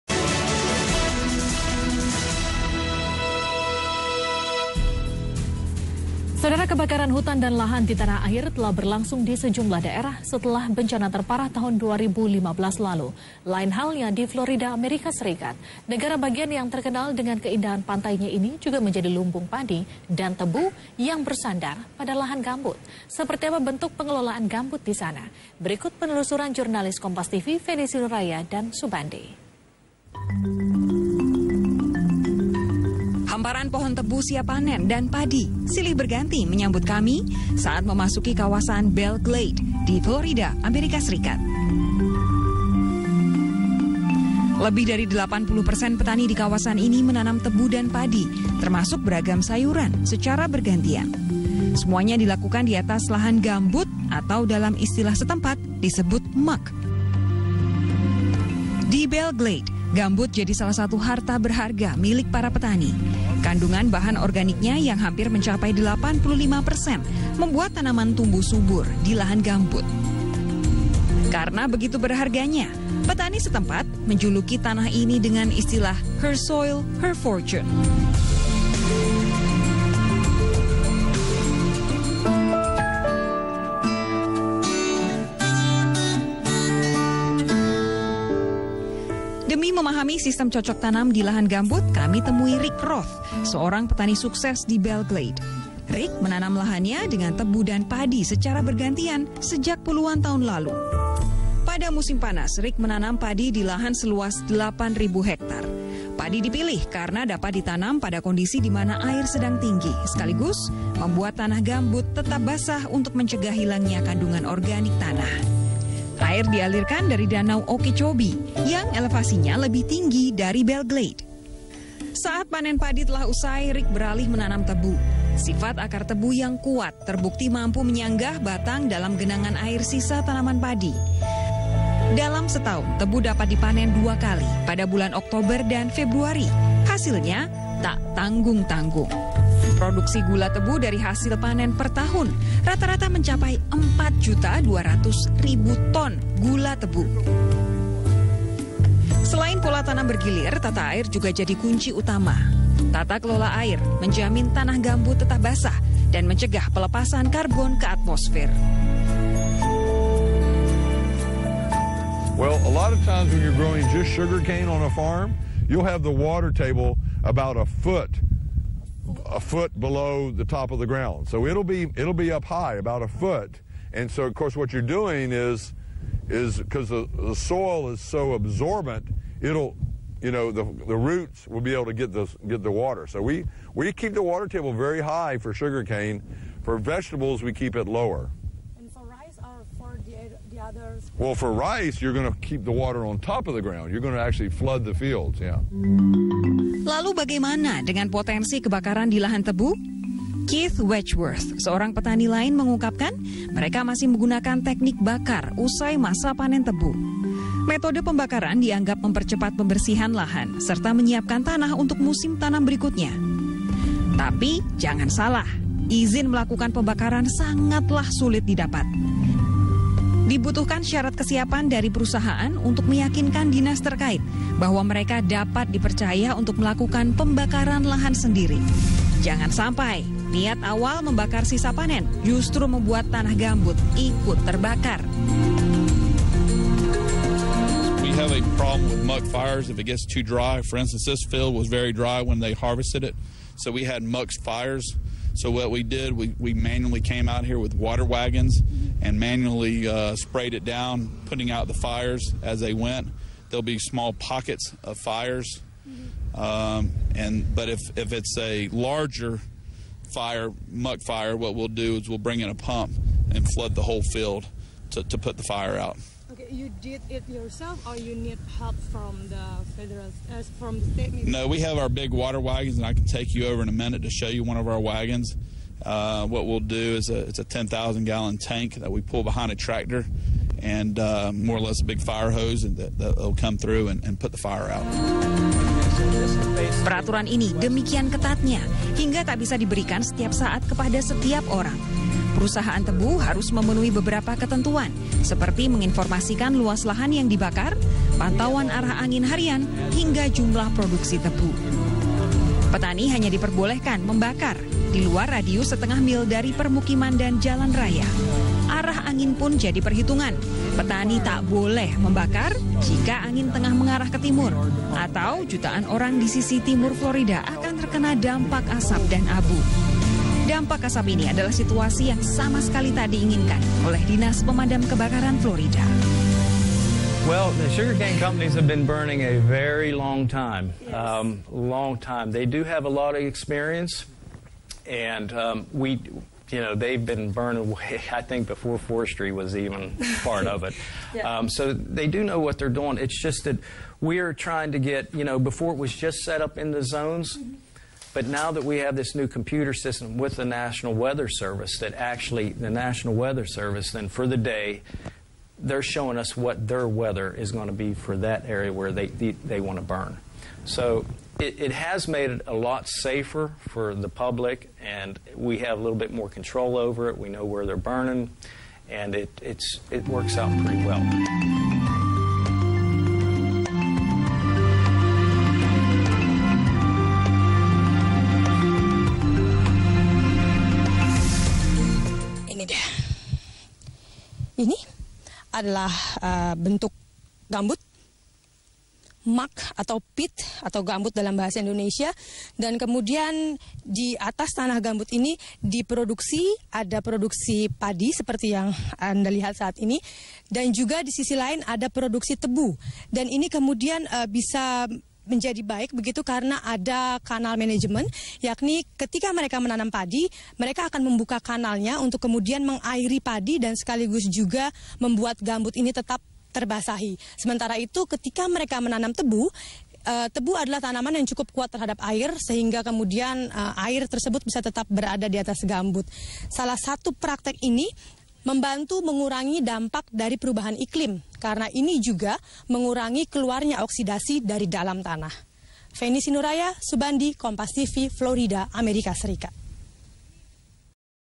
Sistem Pengaturan Lahan Gambut di Belle Glade, Florida, Amerika Serikat - Kompas TV (27/08/16)
Ayo kita intip bagaimana cara mengelola lahan gambut di Florida, Amerika! Negara bagian Florida, Amerika Serikat, menjadi lumbung padi dan tebu yang bersandar pada lahan gambut. Kandungan bahan organiknya yang hampir mencapai 85% membuat tanaman tumbuh subur. Tak heran gambut dianggap harta berharga, tak terkecuali oleh para petani di Belle Glade. Di sana, mereka menanam lahan gambut dengan padi dan tebu secara bergantian selama puluhan tahun. Tata kelola air menjadi kunci kesuksesan para petani. Pada musim panas mereka menanam padi, yang bisa ditanami pada saat kondisi air tinggi sekaligus mencegah hilangnya kandungan organik tanah. Setelah panen padi usai, mereka menanam tebu. Pohon tebu tetap tegak berdiri di lahan basah sisa bertanam padi. Syarat kesiapan yang ketat dibutuhkan untuk meyakinkan instansi terkait bahwa mereka mampu melakukannya. Praktek pola tanam berganti dan tata kelola air yang baik membantu mengurangi dampak dari perubahan iklim karena mengurangi keluarnya oksidasi dari dalam tanah.